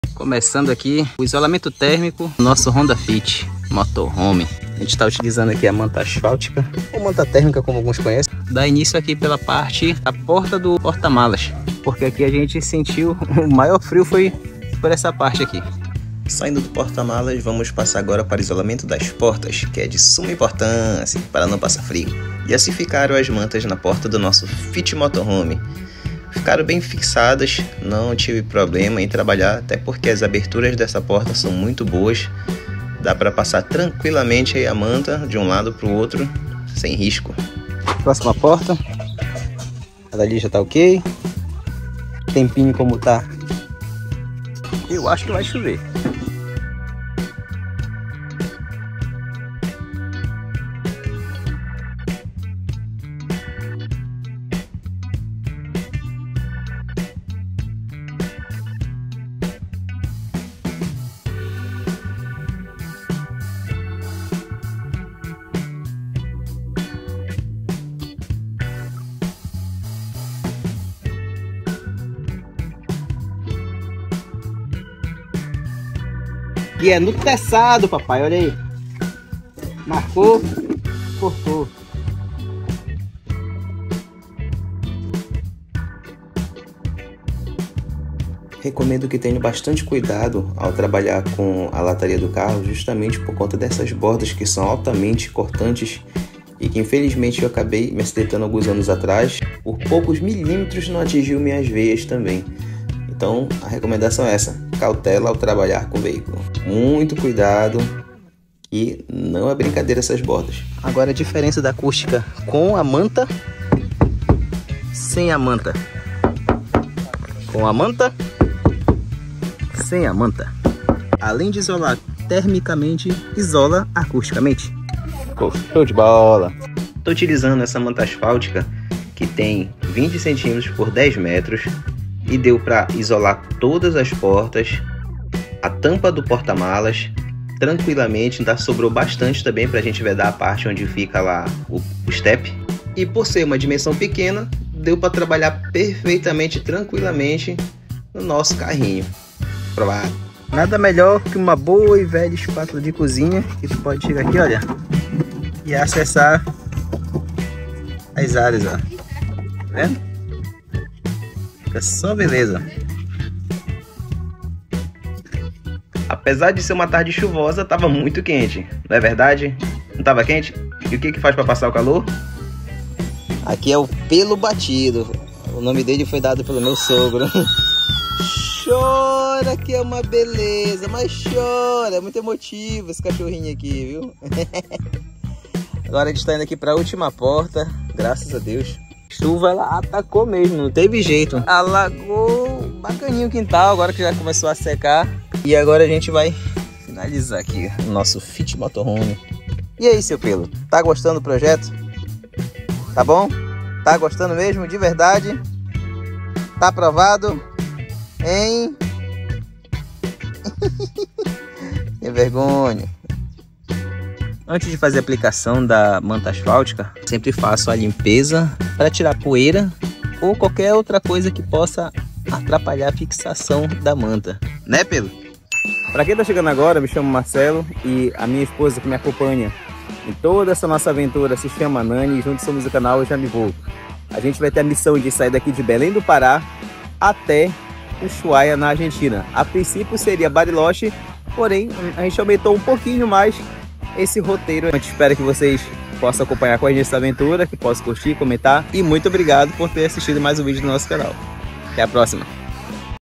ISOLANTE TÉRMICO E ACÚSTICO BARATO | PROJETO BELÉM- ARGENTINA DE CARRO
Neste vídeo ensino como utilizar manta térmica asfáltica para isolar termicamente seu veículo.